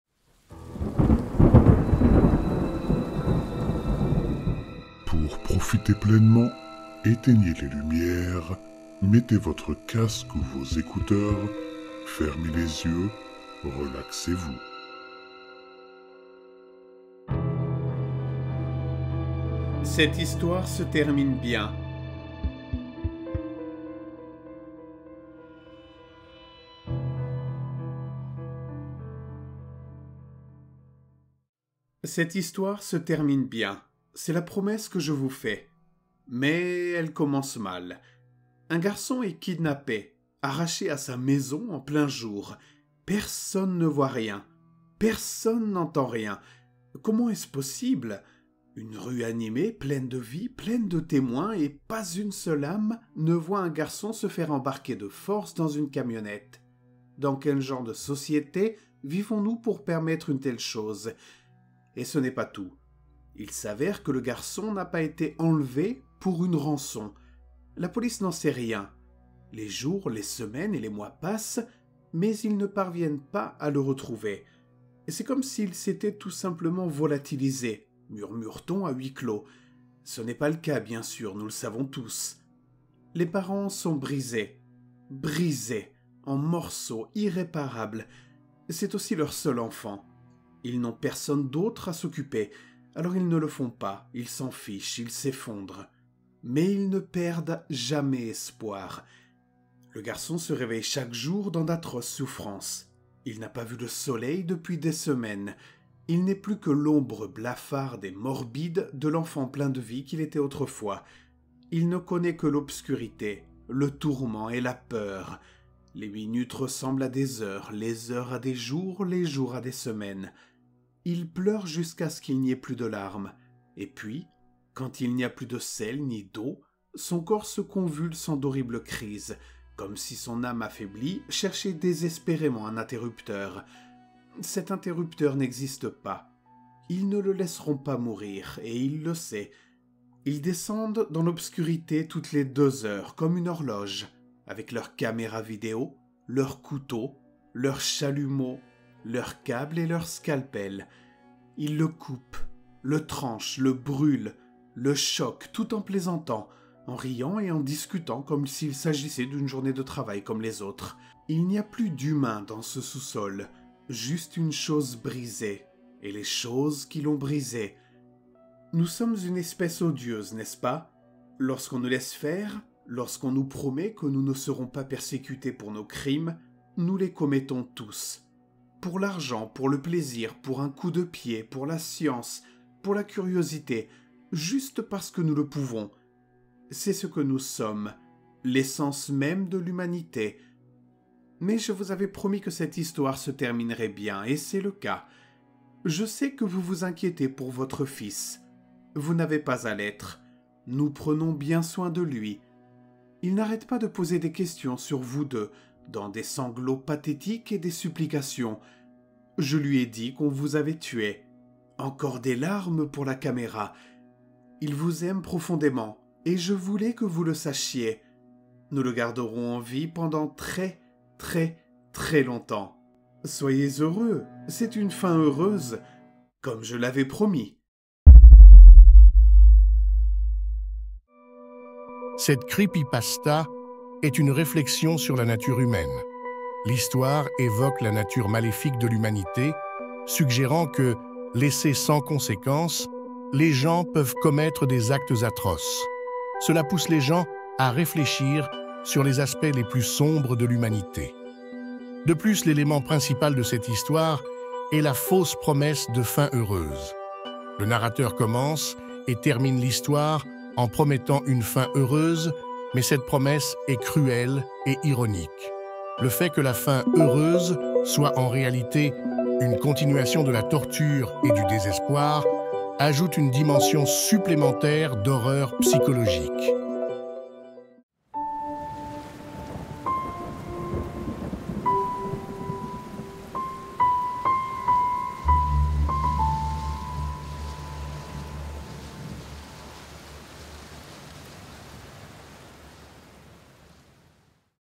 Creepypasta FR - Cette histoire se termine bien (conte horrifique en français)
😱 Faisons-nous peur 😨

"Cette histoire se termine bien"

✍️ Histoire --- Reddit/ShortScaryStories
💡  Catégorie --- Conte Horrifique, Histoire d'Horreur